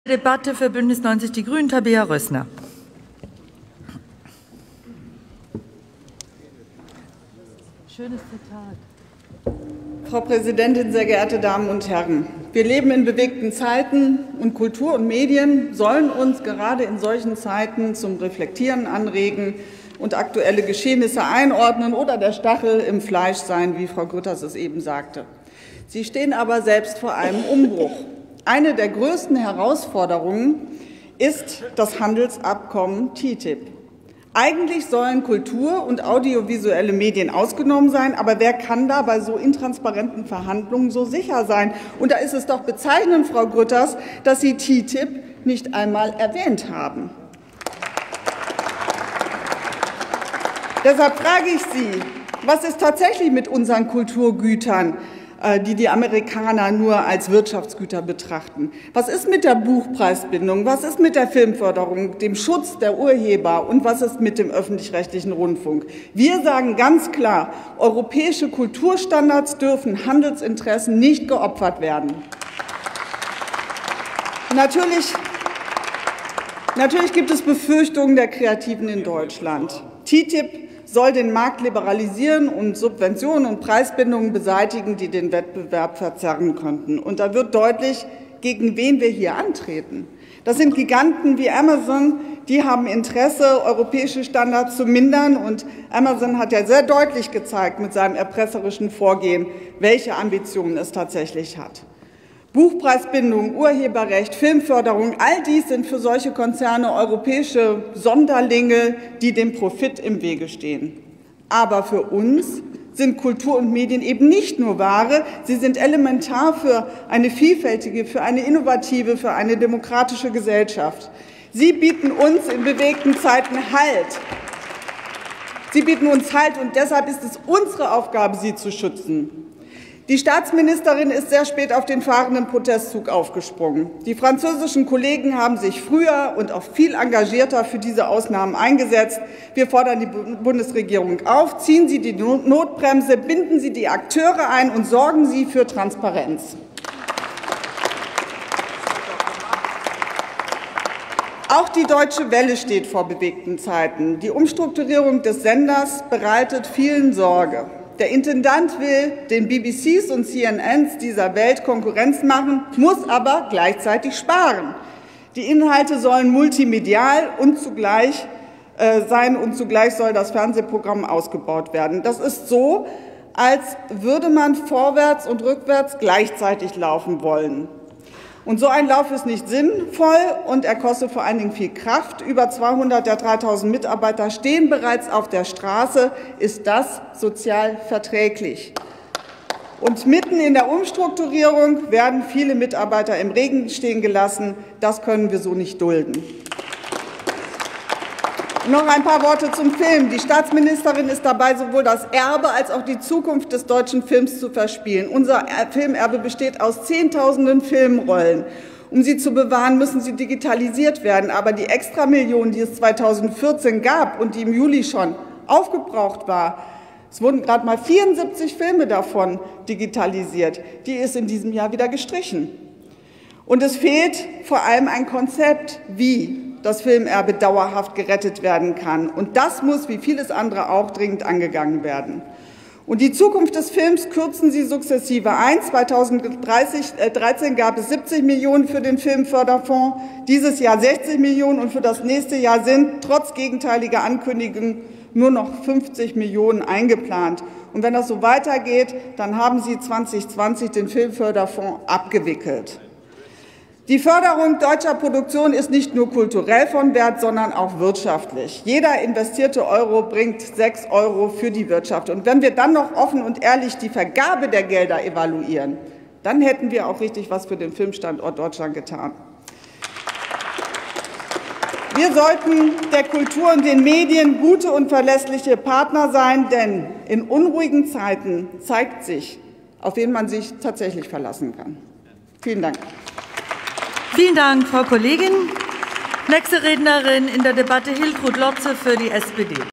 Rede im Bundestag zur Generaldebatte /Haushalt des Bundeskanzleramts 10.09.2014
Rede in der sogenannten Elefantenrunde von Tabea Rößner MdB, Sprecherin für Medien, Kreativwirtschaft und Digitale Infrastruktur der grünen Budnestagsfraktion. In ihrer Rede weist Tabea Rößner darauf hin, welche Unsicherheiten in Bezug auf TTIP bestehen und wie das Freihandelsabkommen europäische Kulturgüter gefährdet. Der Bundesregierung warf Rößner Untätigkeit vor. Weitere Themen der Rede waren die Umstrukturierung der Deutschen Welle, die Filmförderung und die Digitalisierung des Filmerbes.